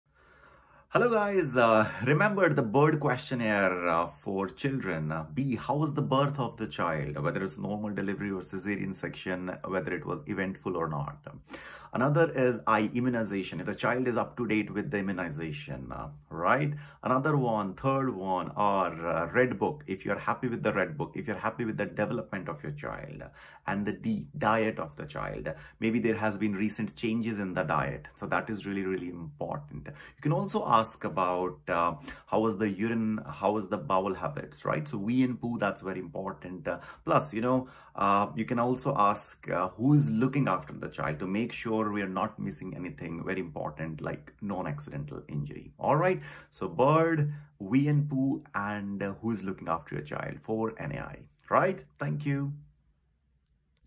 bird questionnaire for Children | Aspire Academy | PLAB2
Remember the bird questionnaire for Children:

- The birth of your child: whether it was normal or via caesarean section, and if there were any complications
- Your child's immunization status: make sure they're up-to-date
- The development of your child: are you happy with their progress and diet?
- Urine and bowel habits: ask about any changes
- Who is looking after your child: to ensure their safety from non-accidental injuries
- Stay informed and proactive about your child's health.